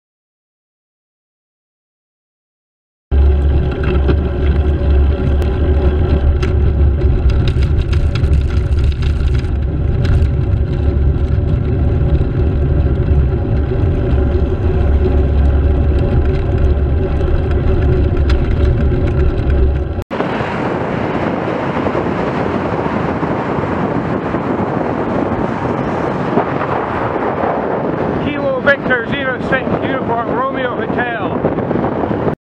KV06URH close pass at speed
Don't know why she indicated because she didn't change lane.

Monday 5th July 2017 approx 08:20